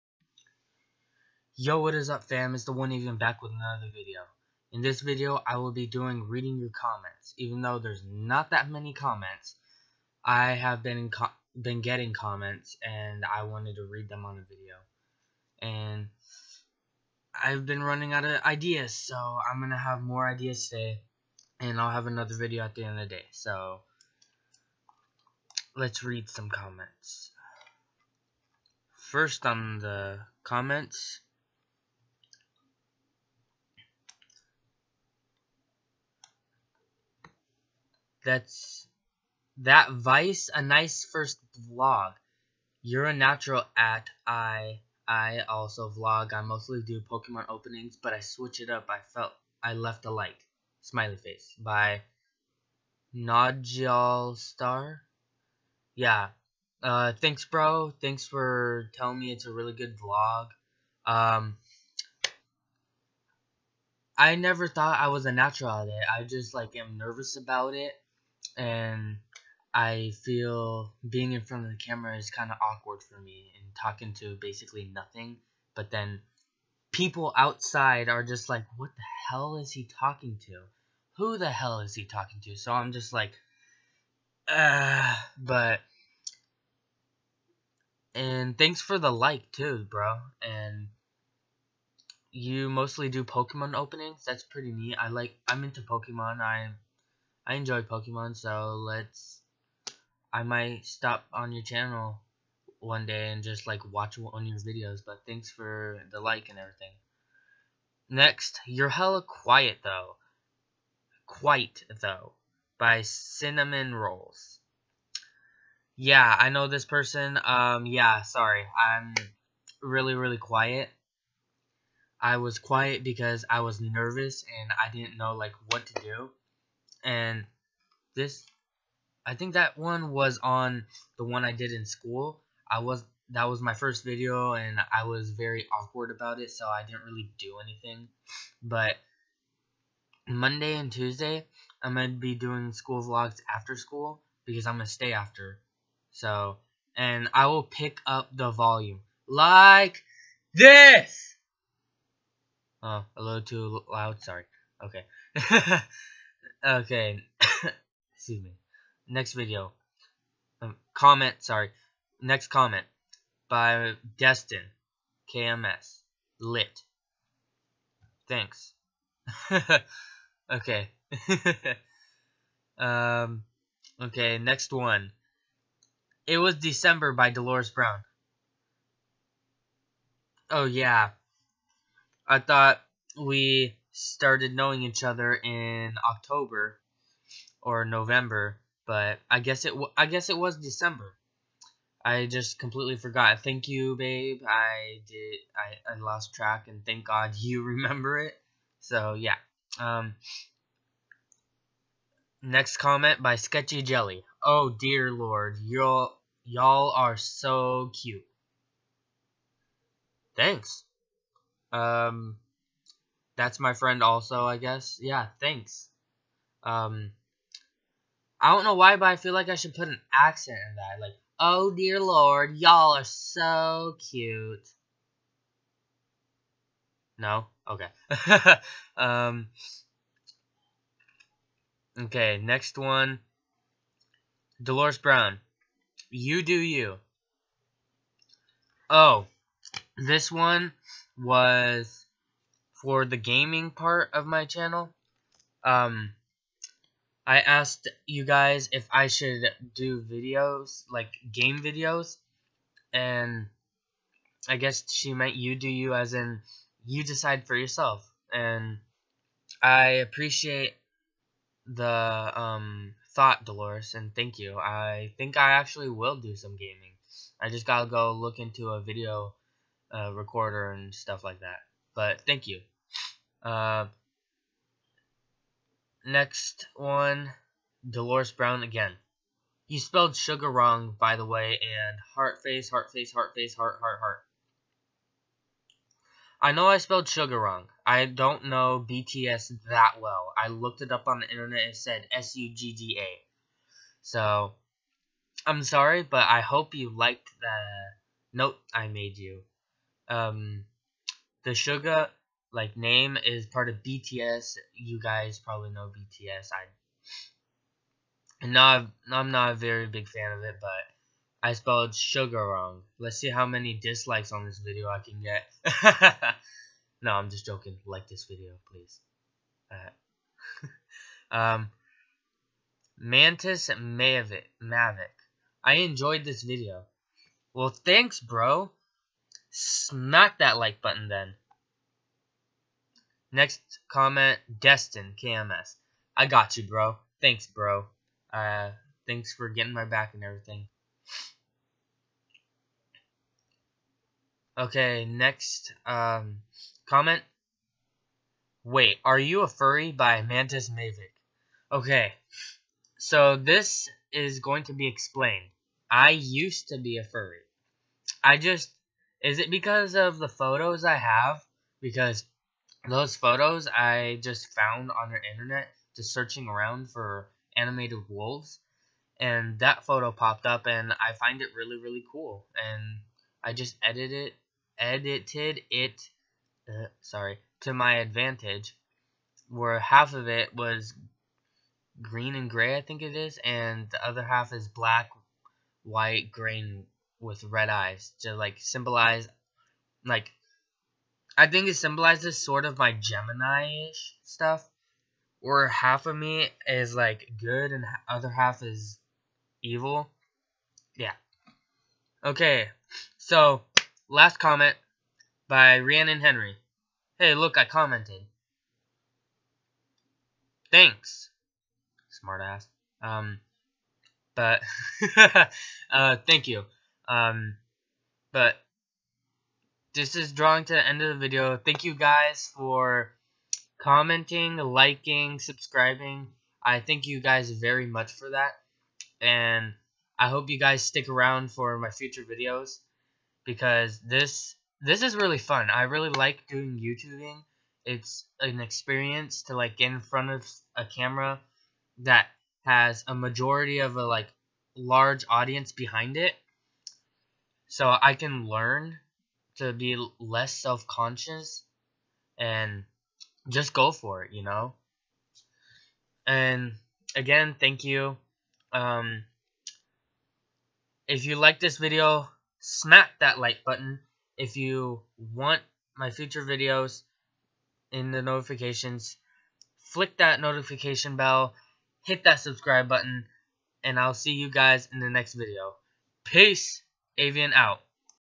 😱Am I A WHAT!?: Reading Your Comments #1
This is my FIRST reading comments video, so I hope you guys enjoy watching it as much as I have enjoyed making it!😜😋😆